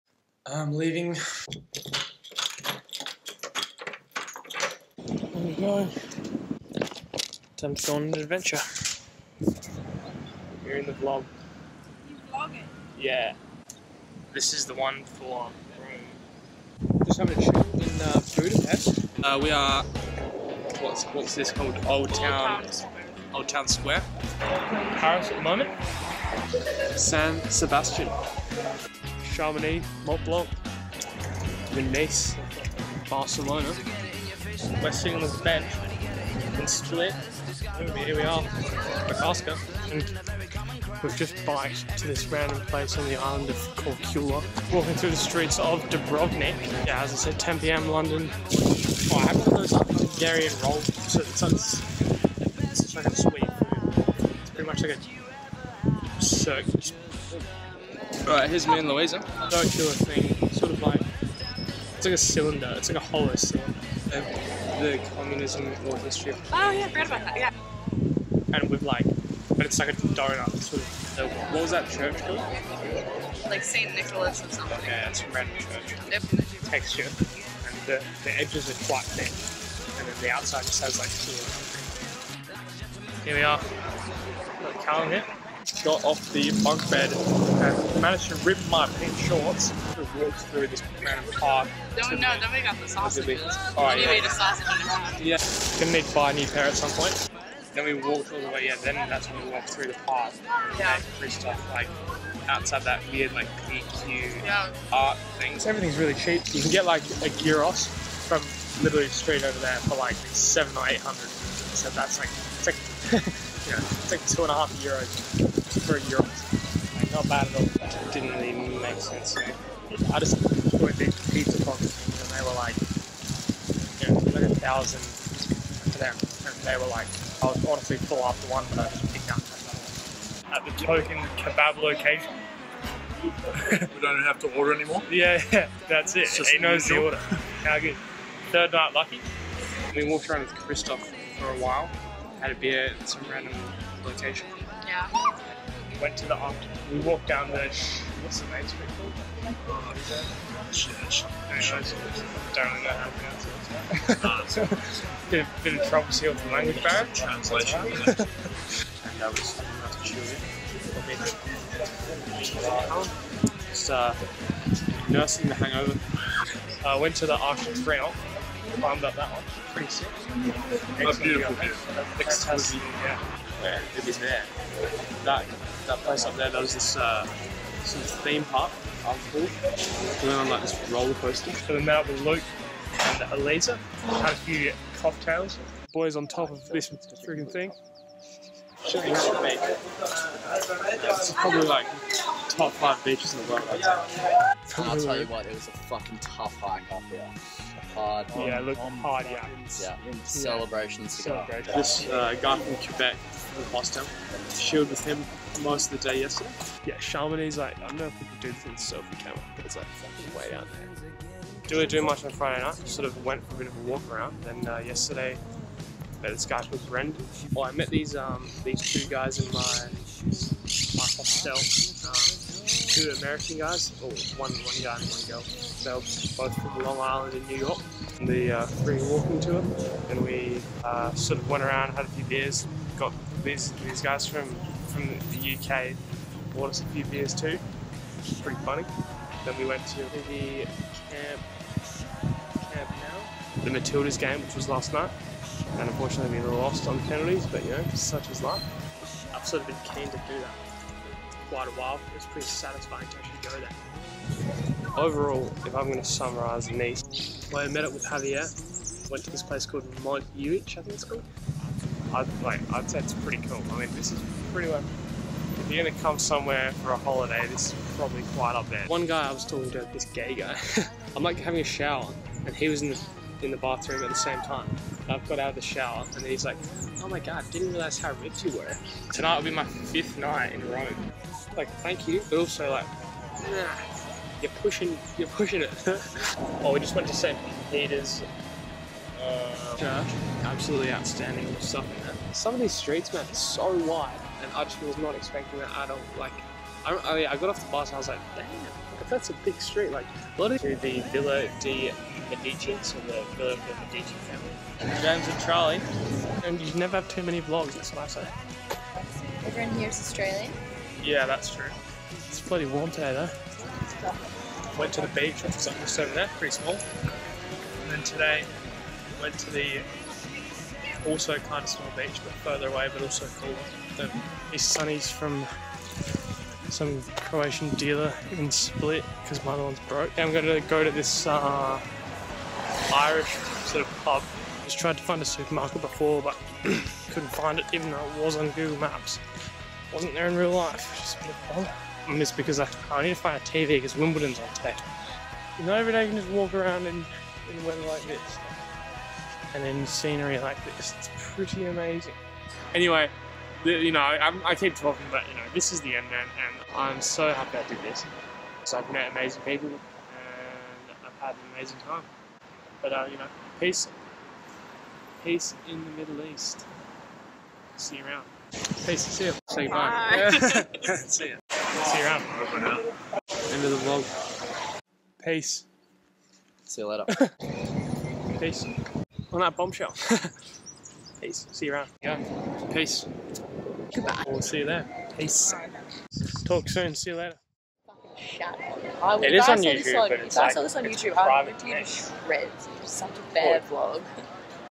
Europe 2019 Vlog
Vlog of approximately 5 weeks travelling Europe after a semester exchange at the University of Sheffield.